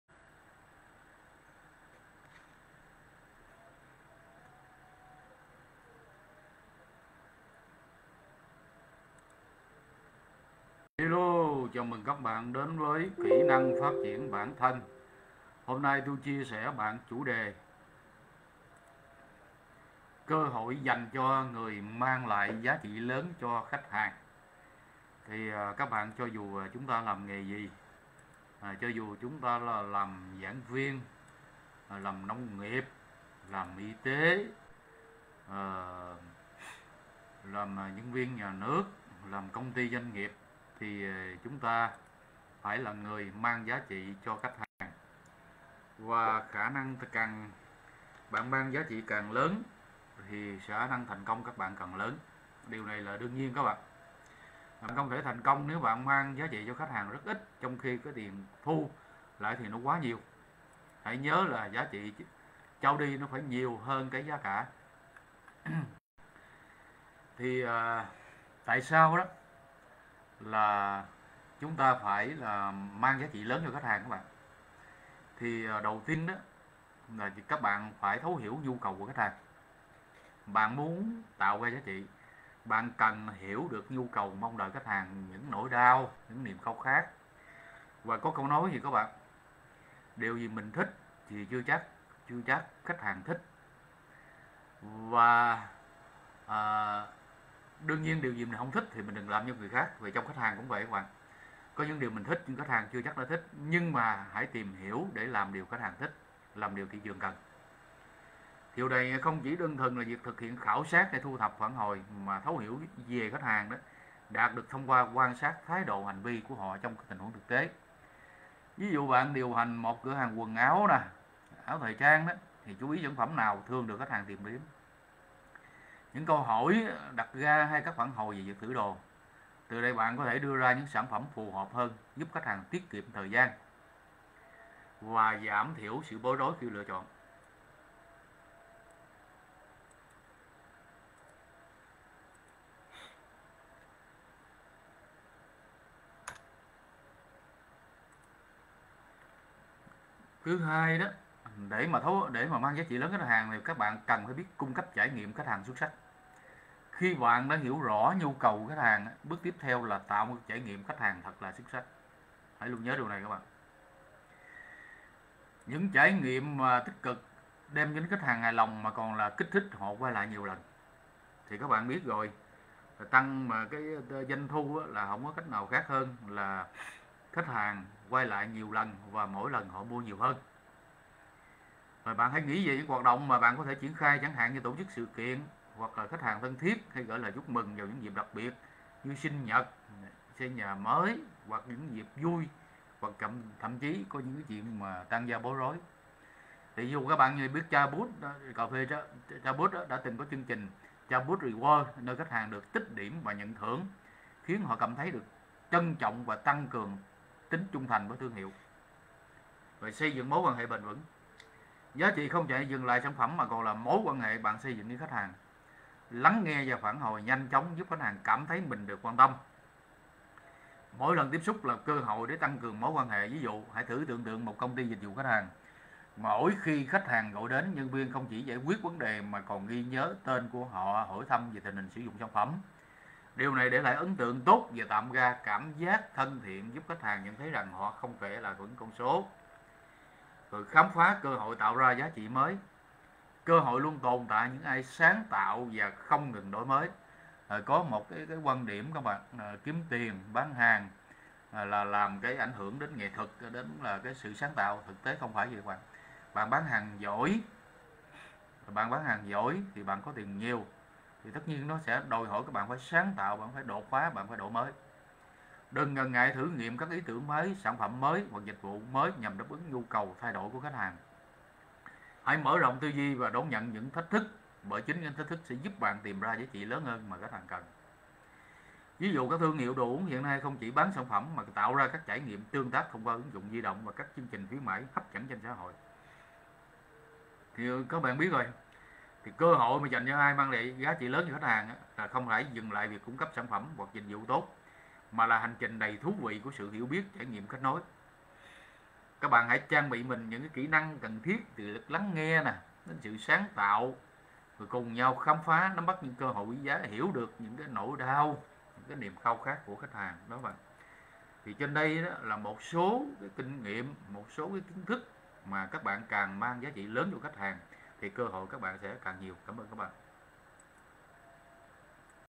CƠ HỘI DÀNH CHO NGƯỜI MANG LẠI GIÁ TRỊ LỚN CHO KHÁCH HÀNG!LÃO GIÀ NGÃ BẢY SÀI GÒN.
Trong một thế giới đầy cạnh tranh như hiện nay, việc xác định và tạo ra giá trị cho khách hàng không chỉ là một phần quan trọng trong chiến lược kinh doanh, mà còn là cơ hội vàng cho những ai có khả năng thấu hiểu và phục vụ khách hàng một cách hiệu quả nhất. Giá trị không chỉ nằm ở sản phẩm hay dịch vụ, mà còn đến từ trải nghiệm khách hàng mà chúng ta mang lại. Vậy làm thế nào để chúng ta có thể nắm bắt cơ hội này và trở thành người mang lại giá trị lớn cho khách hàng?
Thấu Hiểu Nhu Cầu Khách Hàng
Trước tiên, để tạo ra giá trị, ta cần phải hiểu rõ nhu cầu và mong đợi của khách hàng. Điều này không chỉ đơn thuần là việc thực hiện khảo sát hay thu thập phản hồi. Thấu hiểu sâu sắc về khách hàng có thể đạt được thông qua việc quan sát thái độ và hành vi của họ trong các tình huống thực tế. Ví dụ, nếu bạn điều hành một cửa hàng quần áo, hãy chú ý đến những sản phẩm nào thường được khách hàng tìm kiếm, những câu hỏi họ thường đặt ra hay các phản hồi về việc thử đồ. Từ đây, bạn có thể đưa ra những sản phẩm phù hợp hơn, giúp khách hàng tiết kiệm thời gian và giảm thiểu sự bối rối khi lựa chọn.
Cung Cấp Trải Nghiệm Khách Hàng Xuất Sắc
Khi bạn đã hiểu rõ nhu cầu của khách hàng, bước tiếp theo là tạo ra một trải nghiệm khách hàng thật sự xuất sắc. Những trải nghiệm tích cực sẽ không chỉ khiến khách hàng hài lòng mà còn kích thích họ quay lại nhiều lần. Hãy nghĩ về những hoạt động mà bạn có thể triển khai, chẳng hạn như tổ chức sự kiện đặc biệt cho khách hàng thân thiết, hay gửi lời chúc mừng vào những dịp đặc biệt như sinh nhật. Chẳng hạn, Starbucks từng có chương trình “Starbucks Rewards”, nơi khách hàng được tích điểm và nhận thưởng, khiến họ cảm thấy được trân trọng và tăng cường tính trung thành với thương hiệu.
Xây Dựng Mối Quan Hệ Bền Vững
Giá trị không chỉ dừng lại ở sản phẩm mà còn là mối quan hệ mà bạn xây dựng với khách hàng. Lắng nghe và phản hồi nhanh chóng giúp khách hàng cảm thấy mình được quan tâm. Ví dụ, hãy thử tưởng tượng một công ty dịch vụ khách hàng, mỗi khi khách hàng gọi đến, nhân viên không chỉ giải quyết vấn đề mà còn ghi nhớ tên của họ, hỏi thăm về tình hình sử dụng sản phẩm. Điều này sẽ để lại ấn tượng tốt và tạo ra cảm giác thân thiện, giúp khách hàng nhận thấy rằng họ không chỉ là một con số.
Khám Phá Cơ Hội Tạo Ra Giá Trị Mới
Cuối cùng, cơ hội luôn tồn tại cho những ai sáng tạo và không ngừng đổi mới. Đừng ngần ngại thử nghiệm các ý tưởng mới, sản phẩm mới hoặc dịch vụ mới nhằm đáp ứng nhu cầu thay đổi của khách hàng. Hãy mở rộng tư duy và đón nhận những thách thức, bởi chính những thách thức này sẽ giúp bạn tìm ra giá trị lớn hơn mà khách hàng cần. Ví dụ, các thương hiệu đồ uống hiện nay không chỉ bán sản phẩm mà còn tạo ra các trải nghiệm tương tác thông qua ứng dụng di động và các chương trình khuyến mãi hấp dẫn trên mạng xã hội.
Kết Luận
Tóm lại, cơ hội dành cho những ai mang lại giá trị lớn cho khách hàng không chỉ dừng lại ở việc cung cấp sản phẩm hoặc dịch vụ tốt, mà còn là một hành trình đầy thú vị của sự hiểu biết, trải nghiệm và kết nối. Hãy trang bị cho mình những kỹ năng cần thiết, từ việc lắng nghe đến sự sáng tạo, để không chỉ đáp ứng mà còn nâng cao kỳ vọng của khách hàng. Chúng ta hãy cùng nhau khám phá và nắm bắt những cơ hội quý giá này, và trở thành những người mang lại giá trị thực sự cho khách hàng trong mọi lĩnh vực hoạt động của mình.

LÃO GIÀ NGÃ BẢY SÀI GÒN.